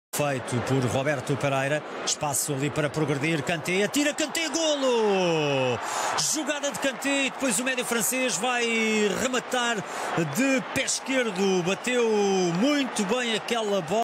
Kante's Goal For Chelsea Against Udinese
Kante played his first pre season game for Chelsea and immediately showed his worth by scoring a beautiful long range goal against Udinese.